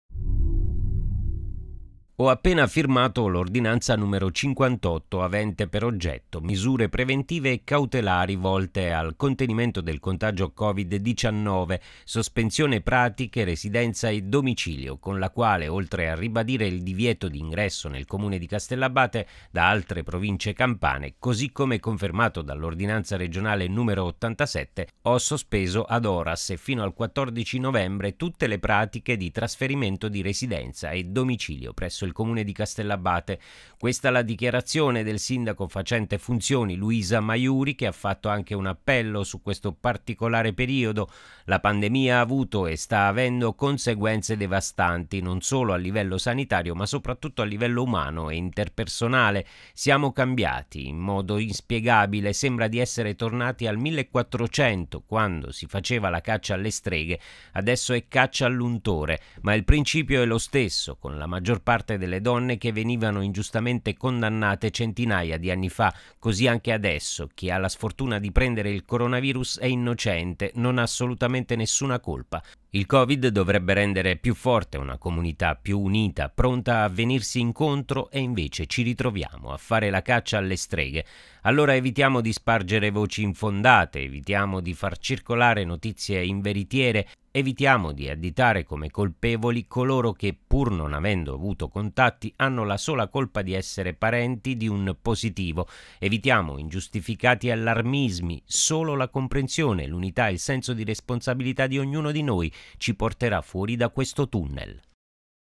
Castellabate, ordinanza sulla sospensione delle pratiche di residenza a domicilio.
Ho appena firmato l’Ordinanza n. 58 avente per oggetto “ Misure preventive e cautelari volte al contenimento del contagio COVID-19.Sospensione pratiche residenza e domicilio “con la quale, oltre a ribadire il divieto di ingresso nel Comune di Castellabate da altre province campane così come confermato dall’Ordinanza Regionale n.87 del 31/10/2020, ho sospeso ad horas e fino al 14 novembre tutte le pratiche di trasferimento di residenza e domicilio presso il Comune di Castellabate.